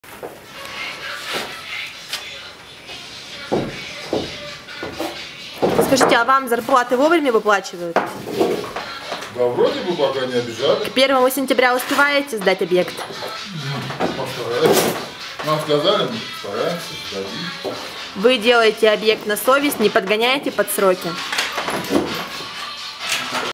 Строитель жилья для погорельцев в Шира: К 1 сентября объект сдадим
Стройка жилья для погорельцев в Шира (Хакасия)